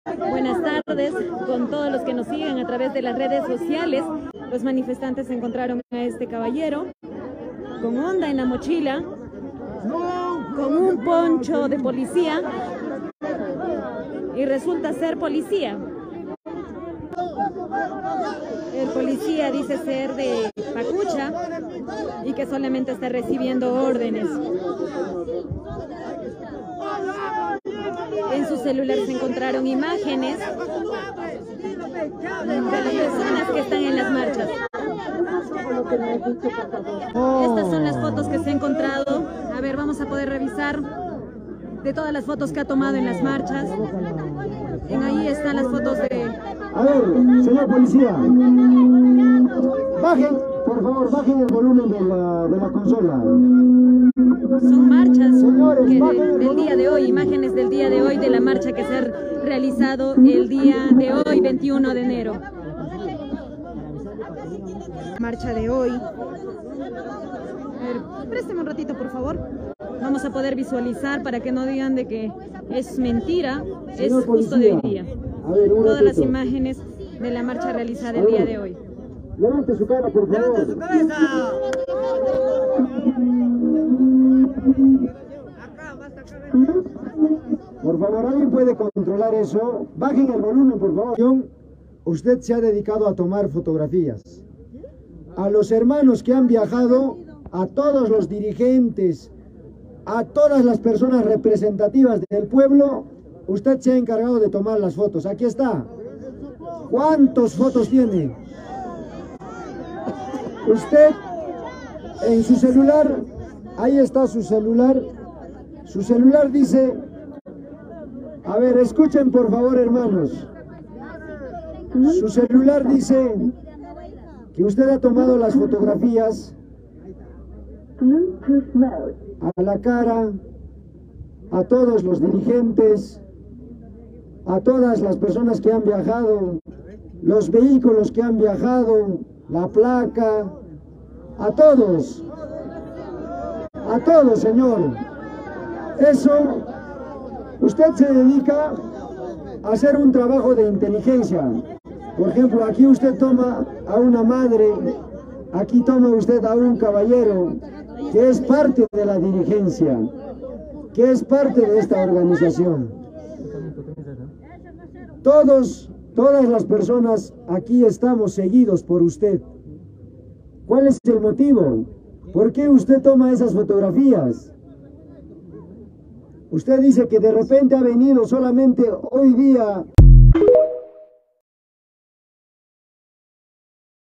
Los Manifestantes capturaron a un Infiltrado que fotografiaba a Dirigentes y Manifestantes.
Los Manifestantes capturaron a un Infiltrado que fotografiaba a Dirigentes y Manifestantes haciendo Trabajo de Inteligencia para la Dictadura de Dina Asesina Boluarte.